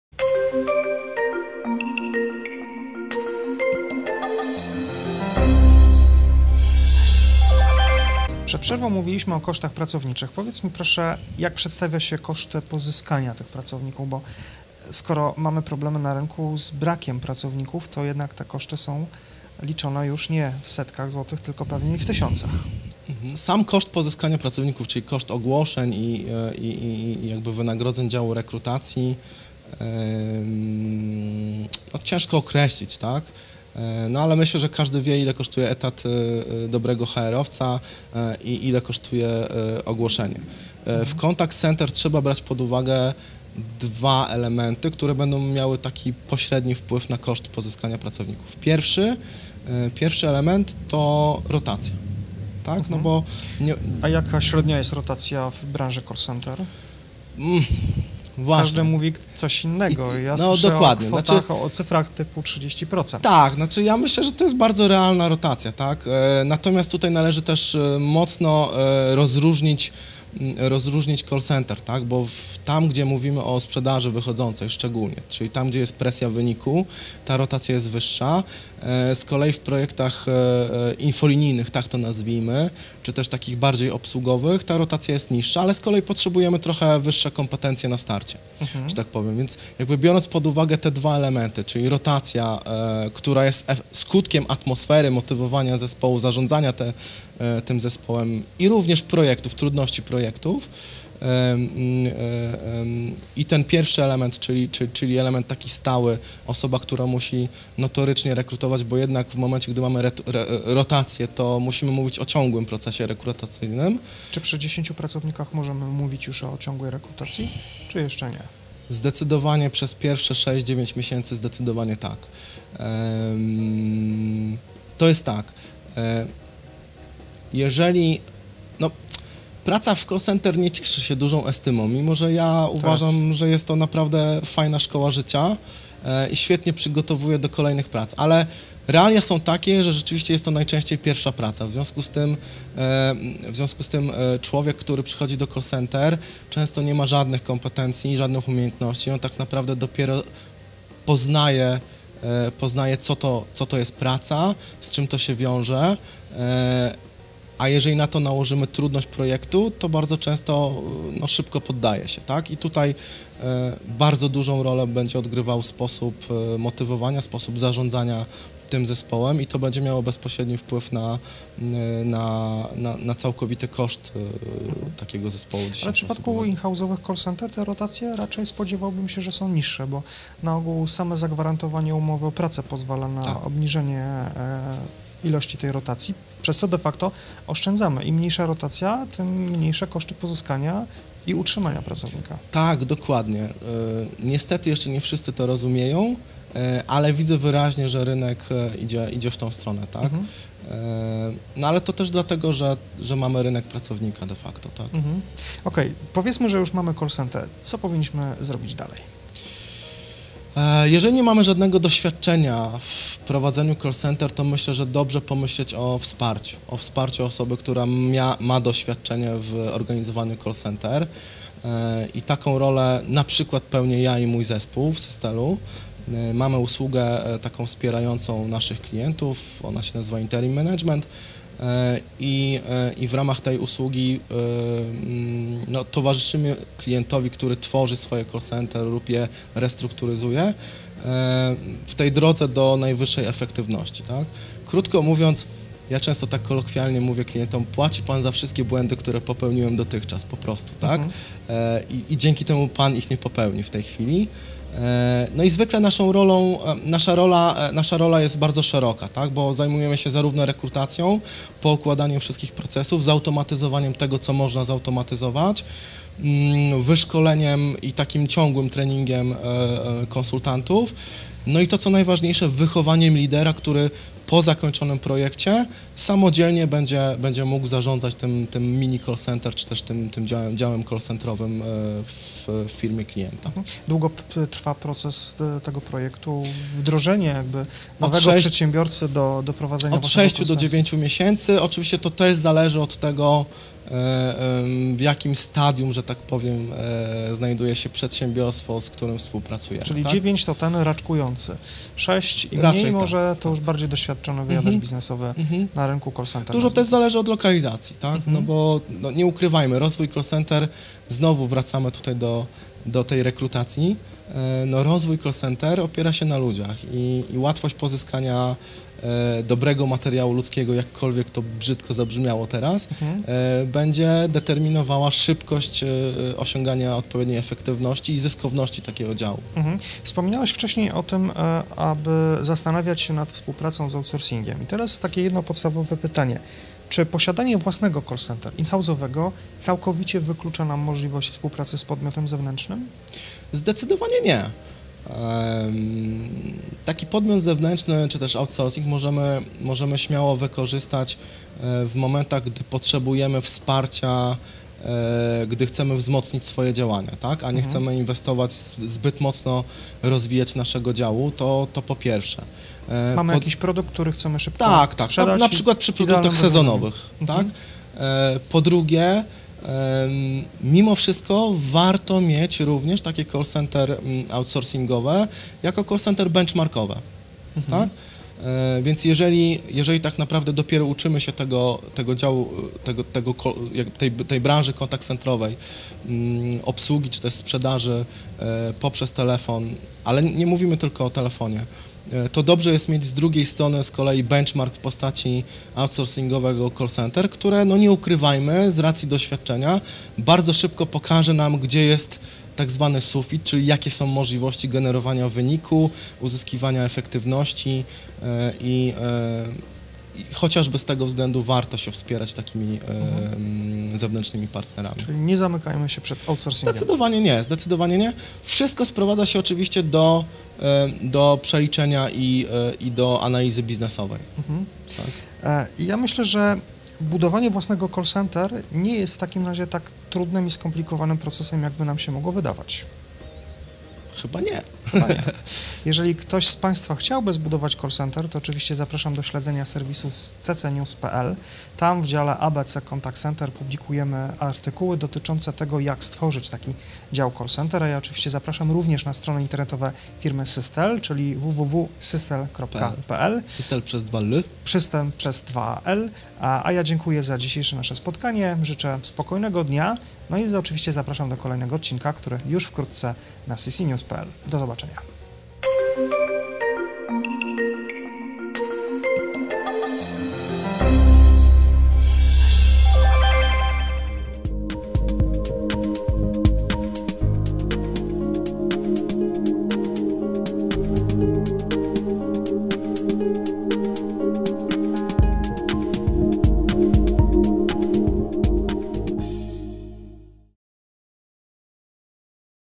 CC TALK #3 | Jak założyć Call Center? Ile kosztuje Call Center? | Jarosław Stępkowski Systell
W ostatniej części rozmowy z Jarosław Stępkowski odpowiada na pytanie: Czy posiadanie własnego contact center wyklucza współpracę z podmiotami zewnętrznymi?